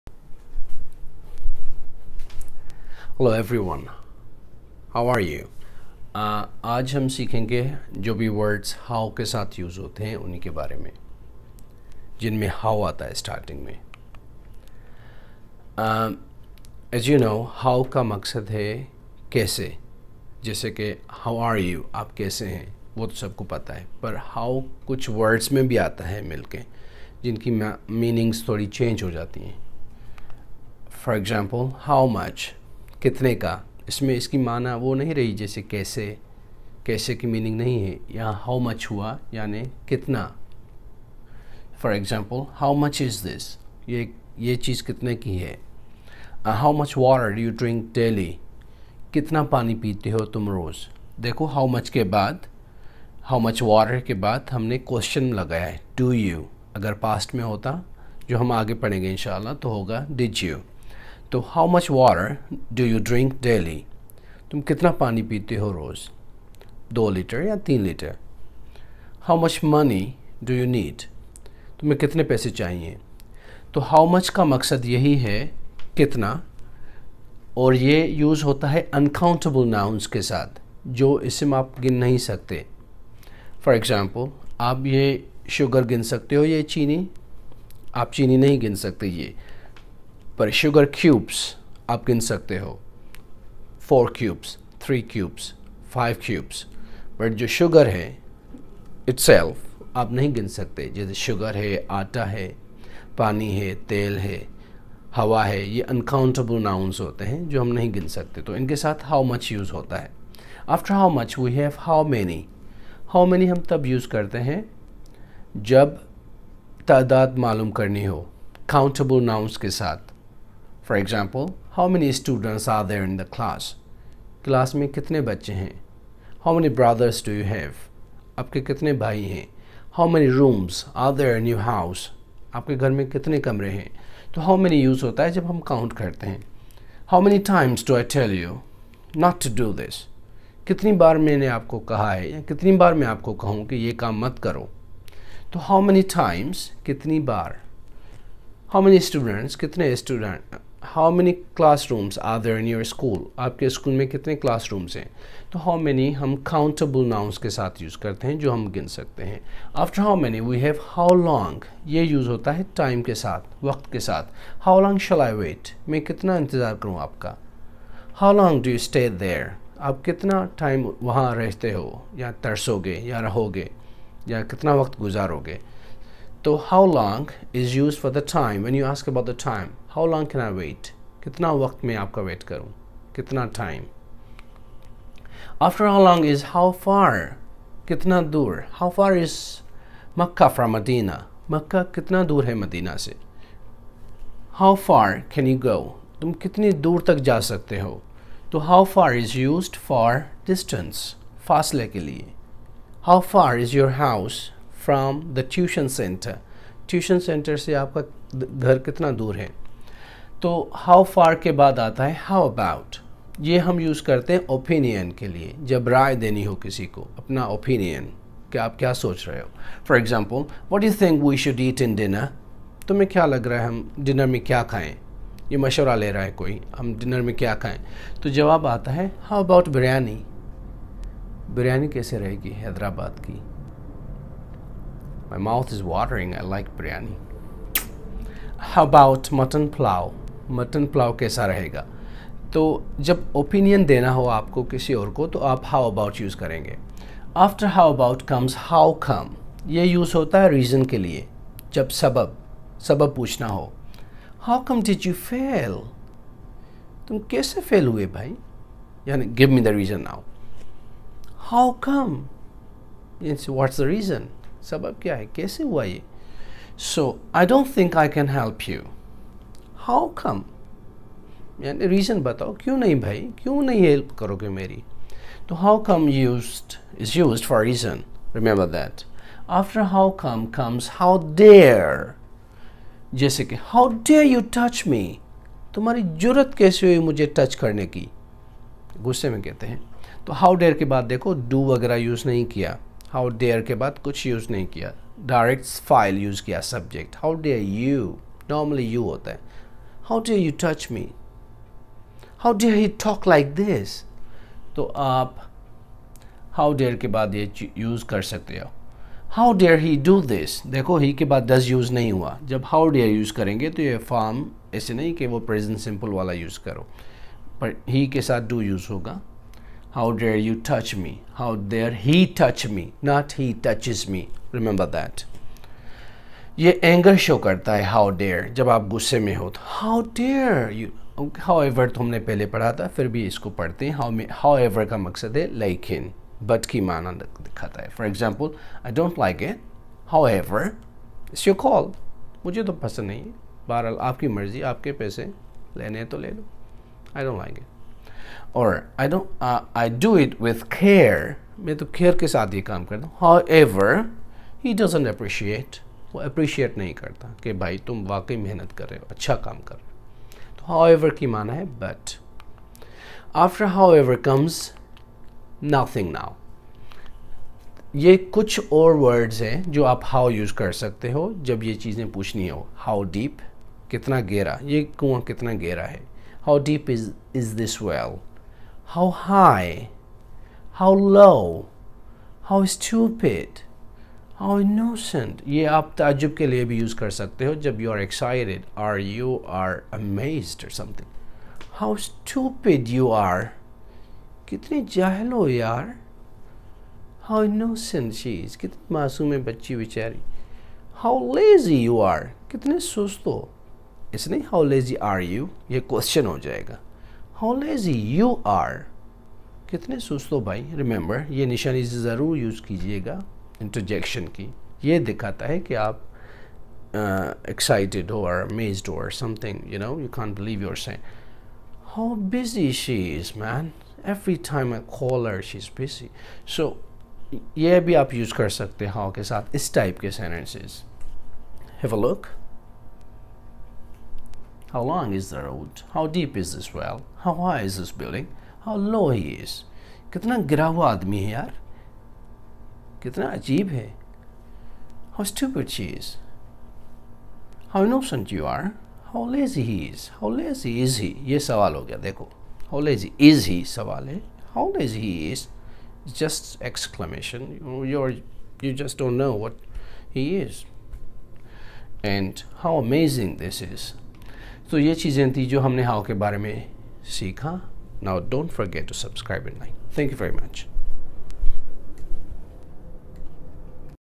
Lesson 18 - How Words हिंदी में स्पष्टीकरणاردو میں وضاحت
However
How many
How much
How long
How far
How deep/ low/high/strange etc

In one sentence, I noticed a typo!

How come you did this!

Not How come did you do this?

Sorry for that... Actually I dont edit the videos.